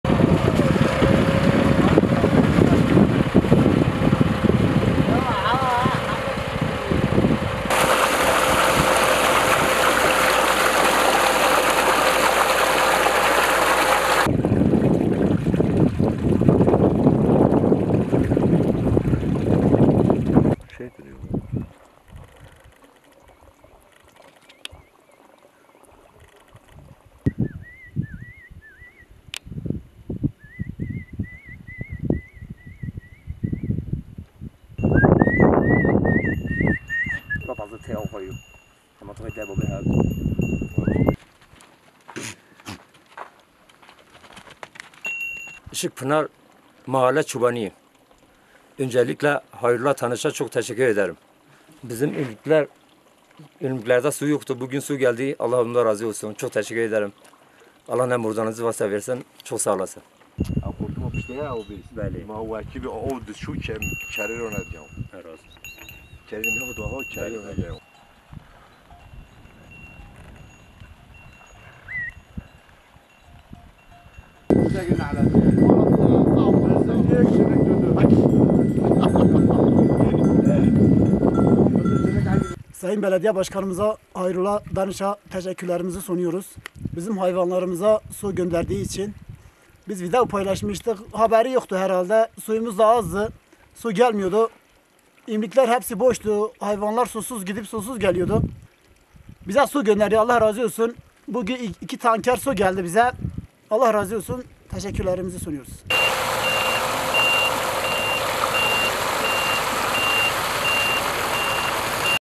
Van'da koyun sürüleri için yaylalara tanker ile su taşınıyor
Van'ın Gürpınar ilçesinde kuraklık nedeniyle su sıkıntısı baş gösterince, koyun sürüleri için yaylalara tankerler ile su taşınmaya başladı.

Van ve çevresinde son zamanlarda artan sıcaklar, birçok baraj, akarsu ve göletin su seviyesinin düşmesine, bazı yaylalardaki su kaynaklarının da tamamen kurumasına neden oldu. Sonbahar aylarına kadar yaylalarda kalan koyun sürüleri su sıkıntısı yaşamaya başlayınca da besiciler, Gürpınar Belediyesi'nden yardım istedi. Bunun üzerine harekete geçen belediye ekipleri, zorlu arazi şartlarına rağmen kırsal yaylalardaki su sıvatlarına tankerler ile su taşımaya başladı.

'MAĞDURİYETE GÖNLÜMÜZ LAZIM DEĞİL'

Gürpınar Belediye Başkanı Hayrullah Tanış, sorumluluk alanlarında olmamasına rağmen sürü sahiplerinin çağrıları sonrası harekete geçtiklerini söyledi. Kuraklıktan kaynaklı sorunun su kaynaklarını verimli ve tasarruflu kullanmakla sağlanacağını belirten Tanış, "İlçemizde tarım ve hayvancılığın daha da gelişmesi için her türlü imkanımızı seferber ediyoruz. Son yıllarda yaşanan bu şiddetli kuraklık maalesef su kaynaklarımızı azalttı. Haliyle hayvanları için su bulmakta güçlük çeken vatandaşlarımız bizden yardım istiyor. Bu durum bizim sorumluluğumuzda olmamasına rağmen vatandaşımızın mağdur olmasına gönlümüz razı değil" dedi.

Kırsal Işıkpınar Mahallesi'nde çobanlık yapan Yunus Baday, su sıvatlarının uzun zamandır boş bulunduğunu ve hayvanlarının zor durumda olduğunu söyledi. Aynı mahallede sürüsünü otlatan Nurtaç Sütçü ise "Allah razı olsun. Su sorunumuzu iletince bölgeye hemen ekipler sevk edildi. Belediye Başkanı'mız Hayrullah Tanış'a ve ekiplere çok teşekkür ediyoruz" diye konuştu.